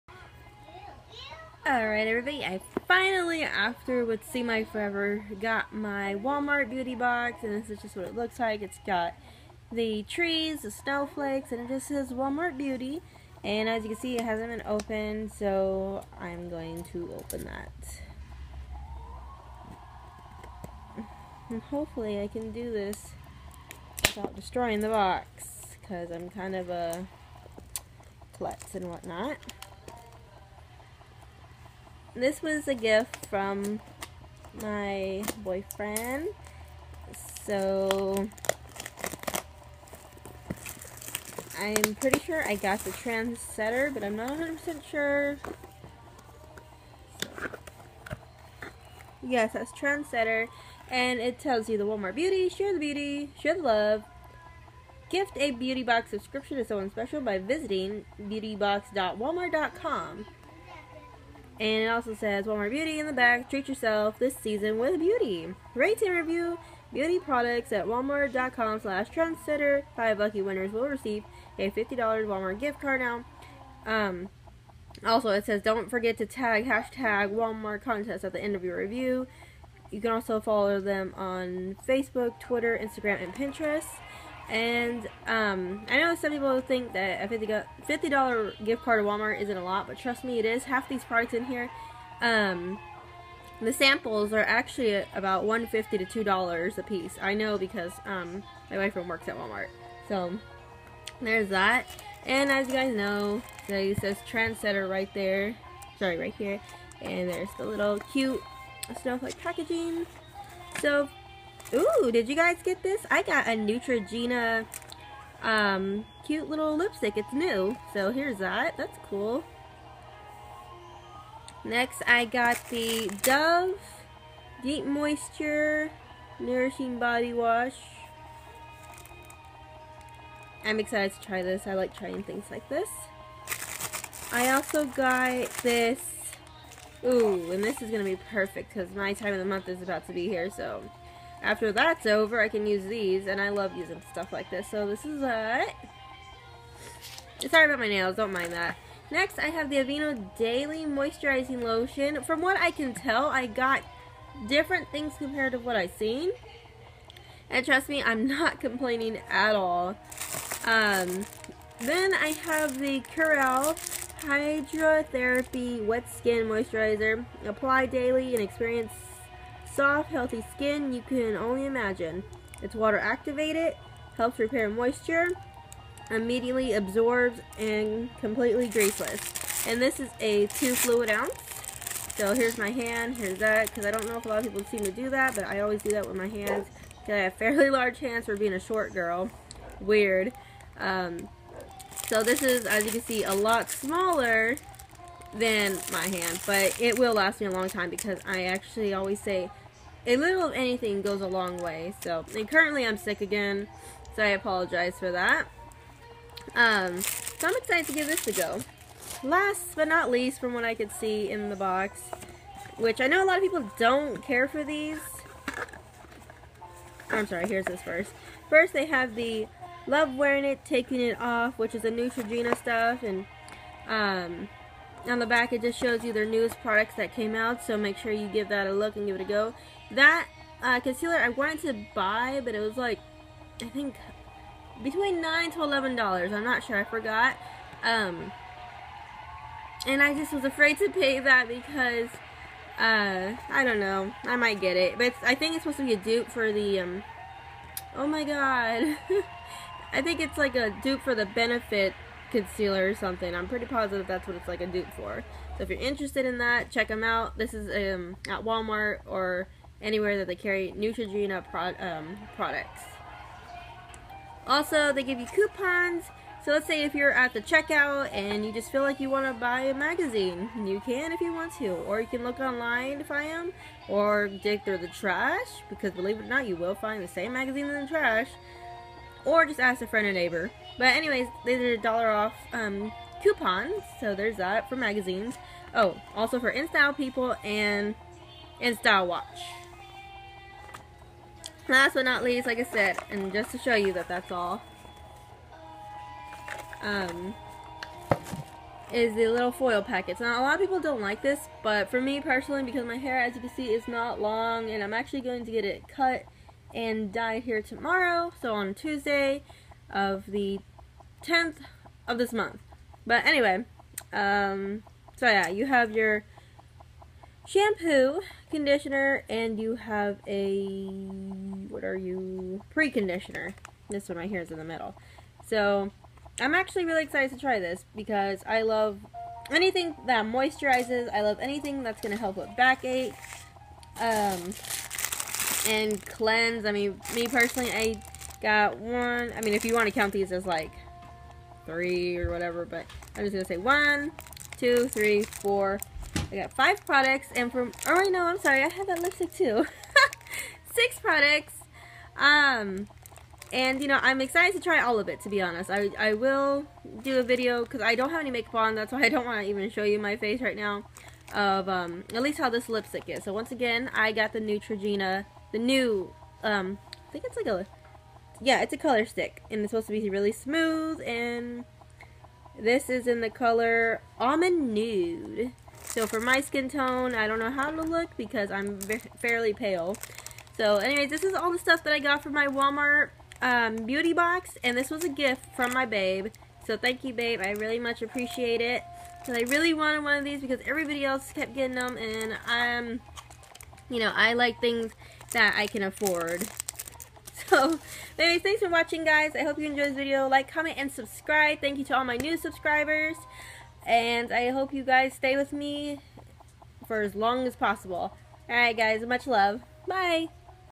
Walmart beauty box winter 2016/2017
So I finally got my box lol and from what I seen my box is different from everyone else's. leave a comment and let me know if you got the same thing!